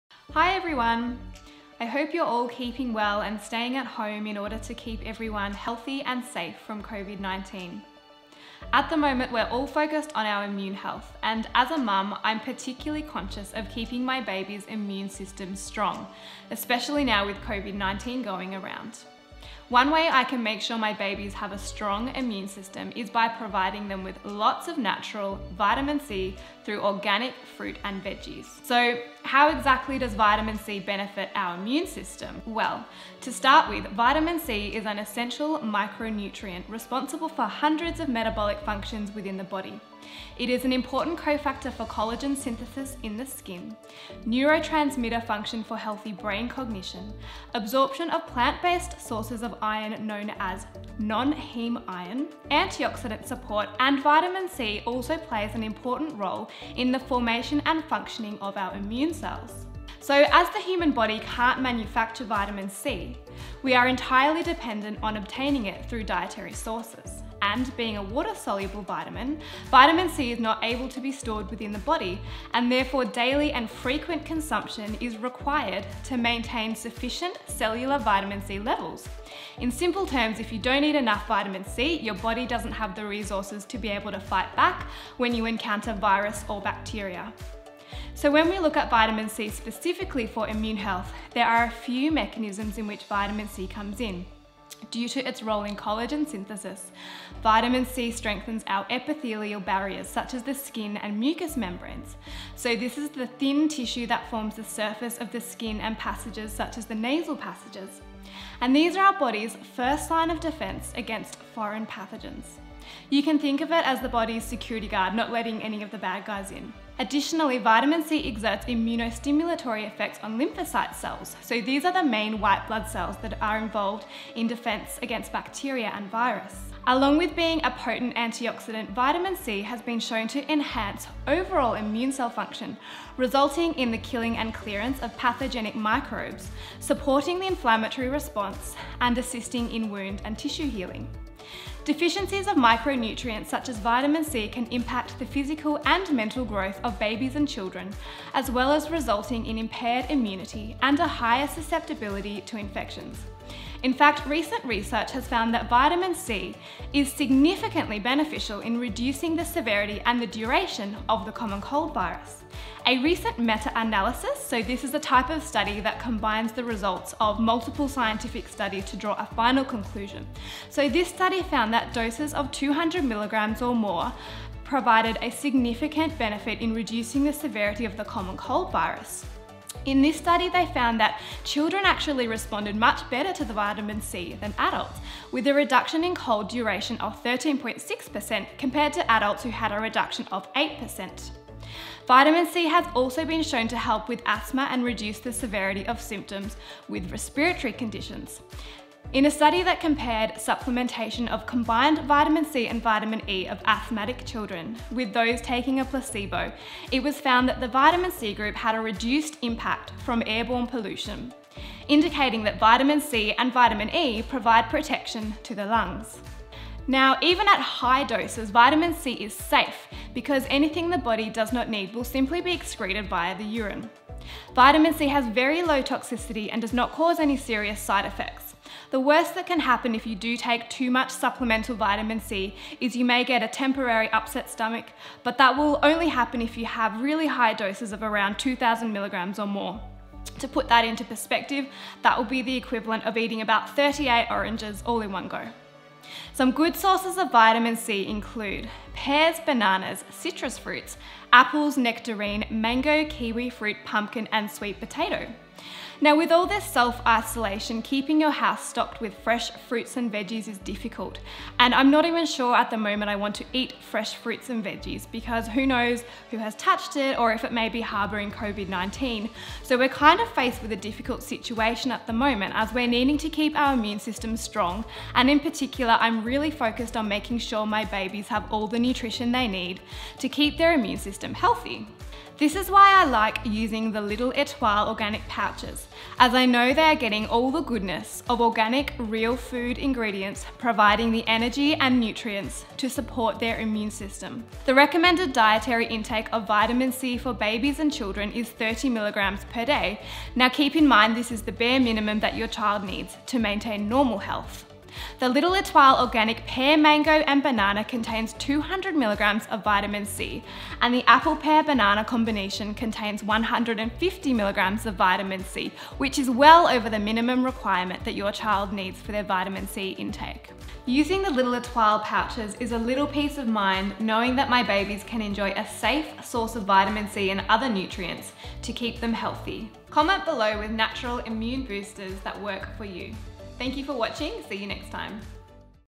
How to boost Child's Immune System during COVID-19?
We’re all focussed on our immune health, and as a nutritionist and a mum, Diana's particularly conscious of keeping my babies immune system strong, especially now with COVID19 going around.
One way we can make sure our babies👶 have a strong immune system is by providing them with natural vitamin C through organic fruit 🍏🍊🍓and veggies🥦🥬 .
How exactly does vitamin c benefit our immune system? please watch  our video ☝